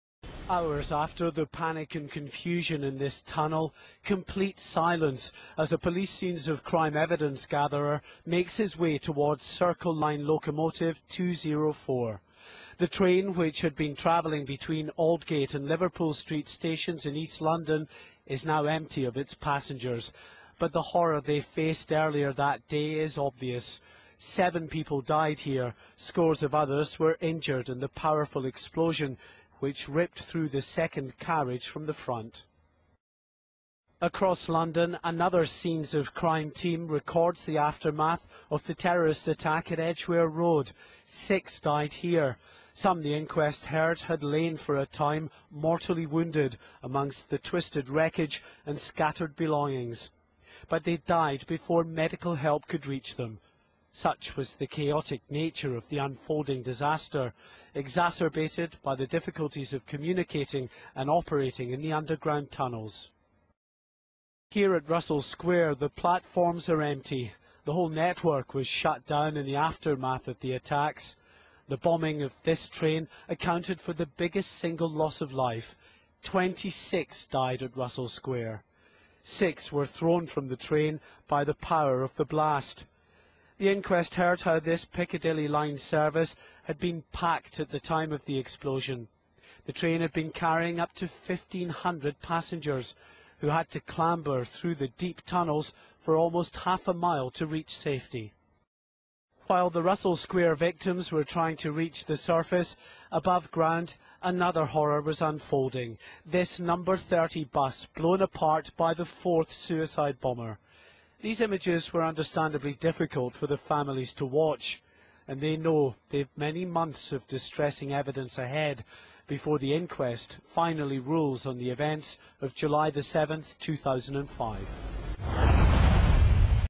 New Video Released Of 7/7 Bomb Aftermath
October 12th 2010:
The inquest into the deaths of the 7/7 victims has seen new video footage of the aftermath of the attacks.

The devastation caused by the bombs was described by the QC as "akin to a battlefield," as the inquest heard how the victims died.